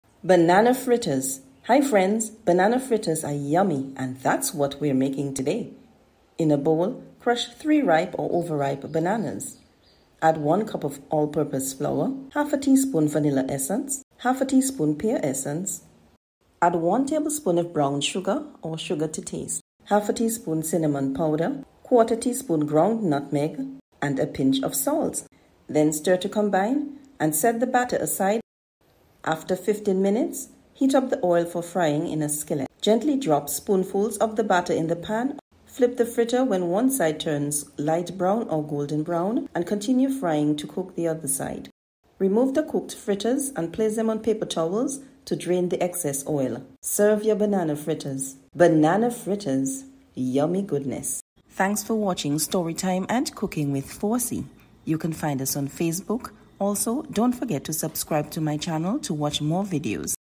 How to Make Banana Fritters | Dominican Banana Fritters Recipe
Banana Fritters are yummy! These banana fritters are lightly flavored with nutmeg and cinnamon to make a delicious side for breakfast, lunch or dinner. That's right! They can be eaten at any time. The kids love it - your entire family and guests will too. At Story time and Cooking with Forsi, we share stories and cook food that is easy and flavorful.